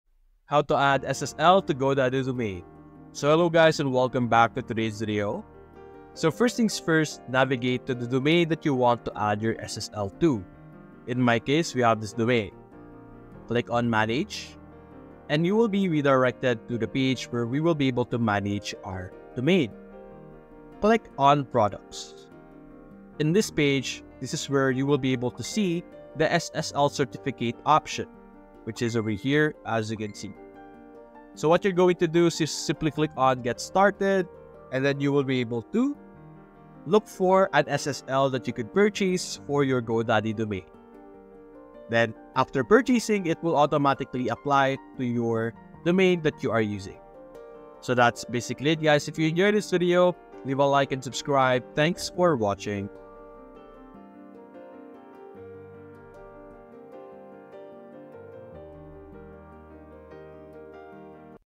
How to Add SSL to Godaddy Domain (Best Method)
Learn the best method for adding SSL to your GoDaddy domain in this step-by-step tutorial. Keep your website secure with SSL!